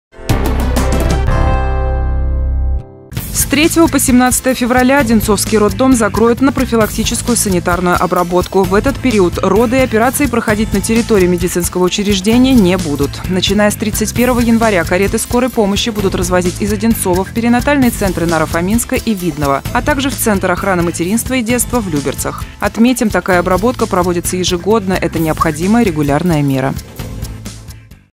Вниманию будущих мам
С 3 по 17 февраля Одинцовский роддом закроют на профилактическую санитарную обработку.